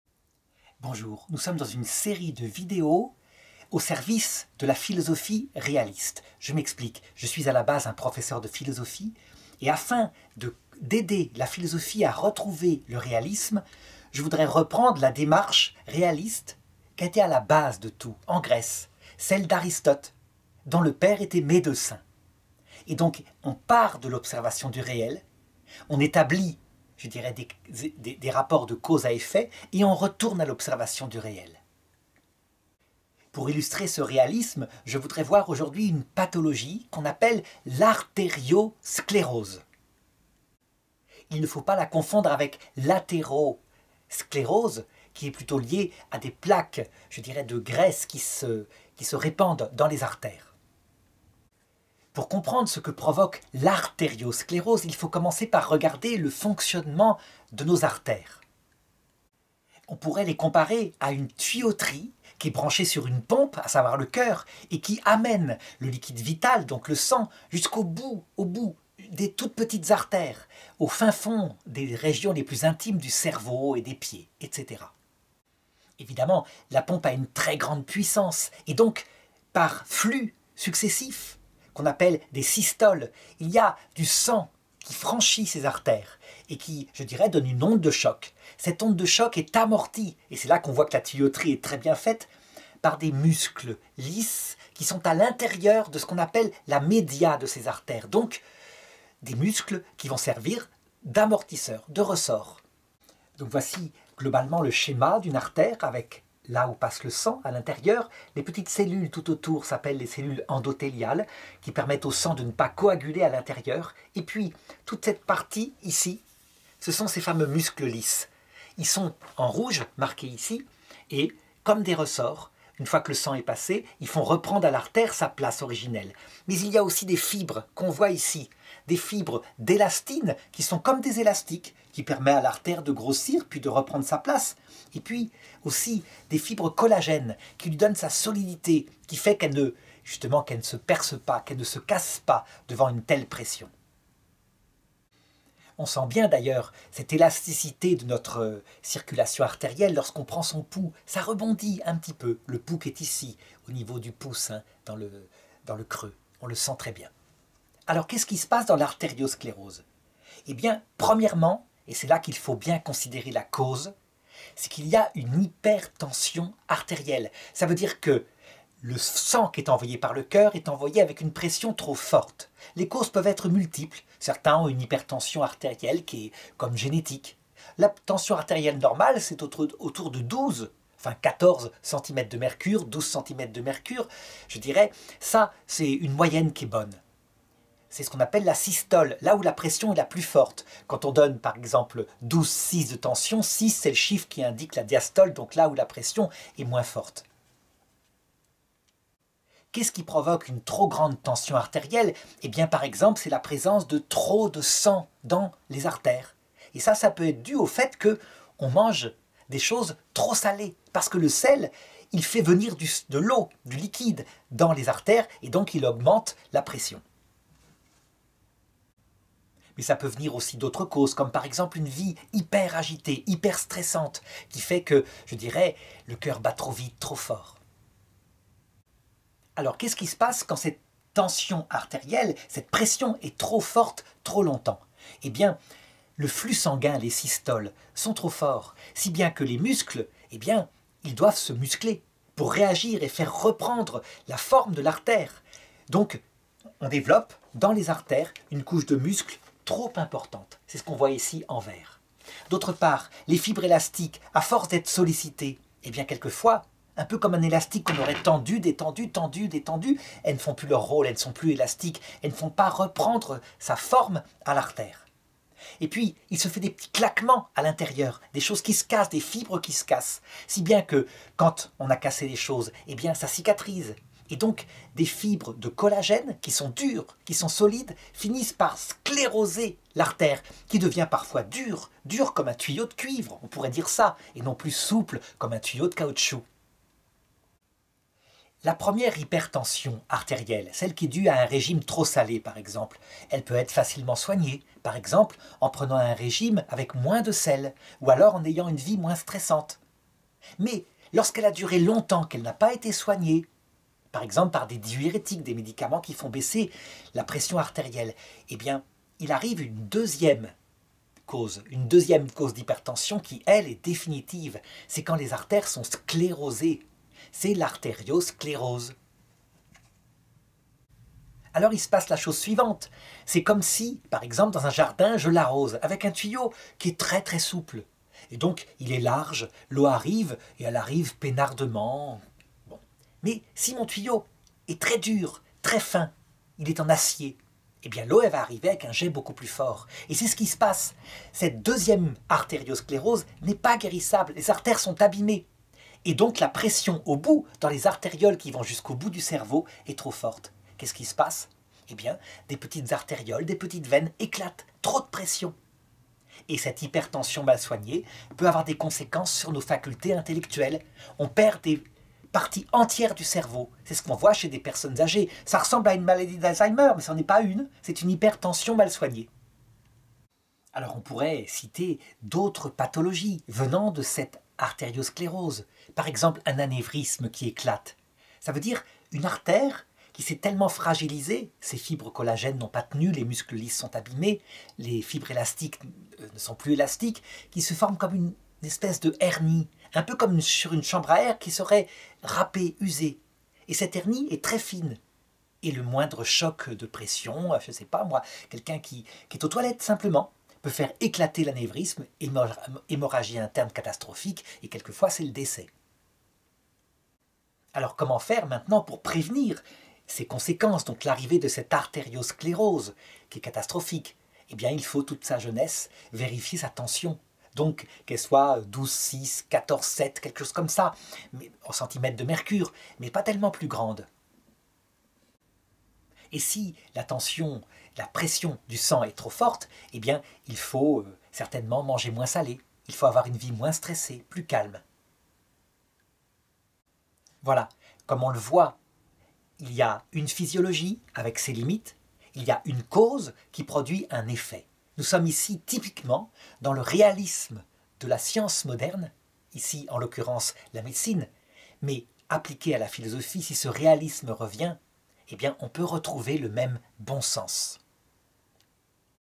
Biologie 3 ─ L’artériosclérose, exemple de biologie humaine (8 mn)
En remerciement au Docteur Pattyn, professeur à la Haute Ecole Condorcet de Charleroi, 2017

Manifester le réalisme de cette partie de la philosophie qu’est la science moderne.
Montrer le mécanisme de l’artériosclérose, source de nombreux arrêts cardiaques dans le monde contemporain. L’hypertension primaire, source d’une hypertension secondaire.
PHILOSOPHIE DU VIVANT 4 ─ QUELQUES ASPECTS DE LA BIOLOGIE HUMAINE
Une philosophie réaliste visant à la lecture de la théologie de saint Thomas d’Aquin.
Par Arnaud Dumouch, 2017.
Les œuvres complètes en français de saint Thomas d'Aquin.